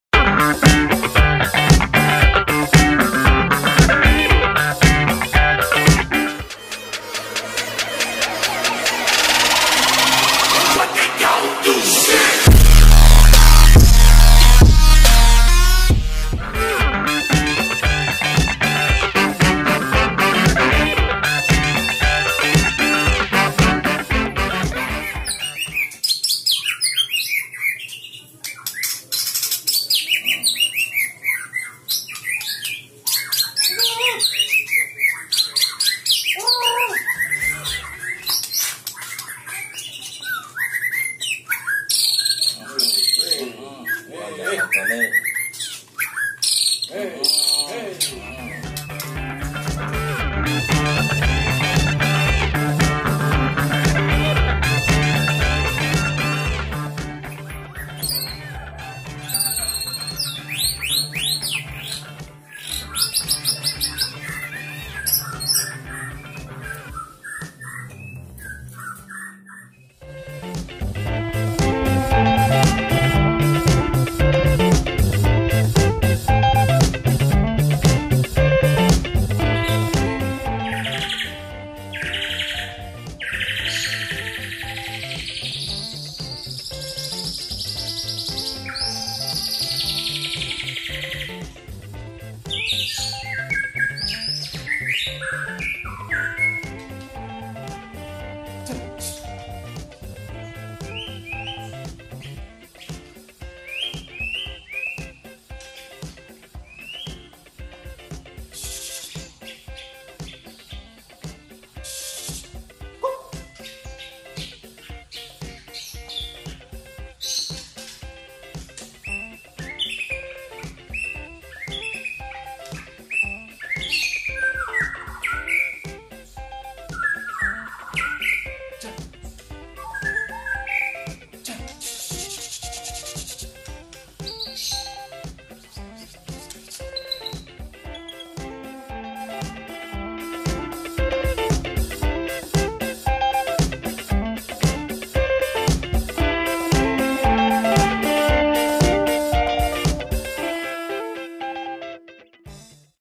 "SEGAWAN" Murai Batu Mewah full isian Mr. Tower
MB "SEGAWAN" Menjadi Mewah Berkat Polesan Tangan Dingin dari Seorang Tower Salah satu Pentolan dr Team Cemara "